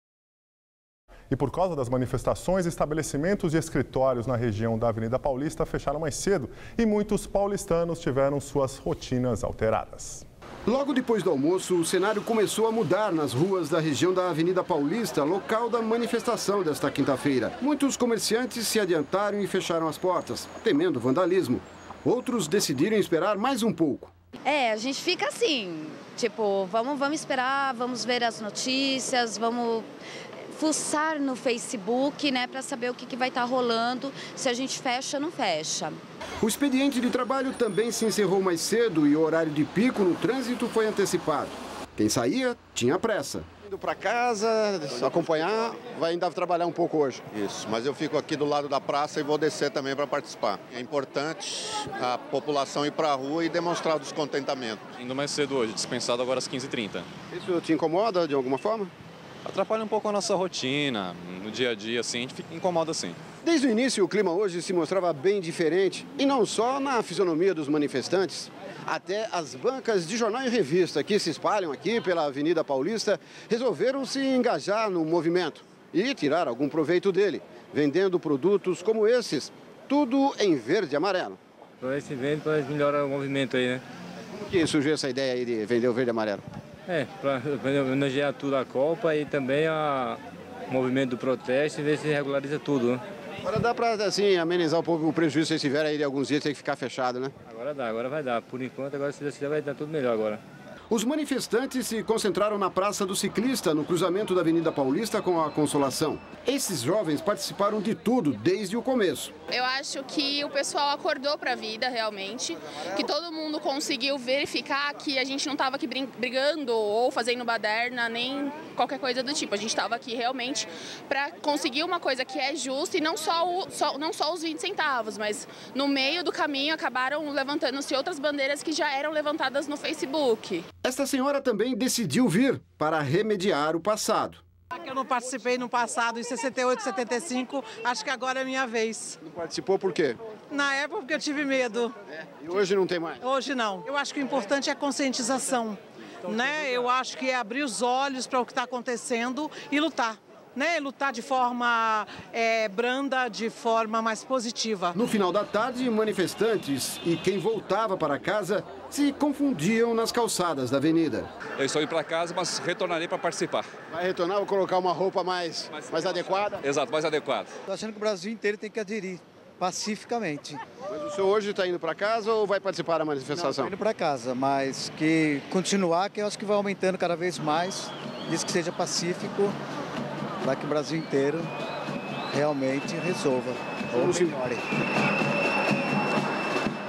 Dia tenso na região da av. Paulista
E por causa das manifestações, estabelecimentos e escritórios na região da avenida Paulista fecharam mais cedo, e muitos paulistanos tiveram suas rotinas alteradas.

Repórter: José Donizete

Tema: Protesto/manifestação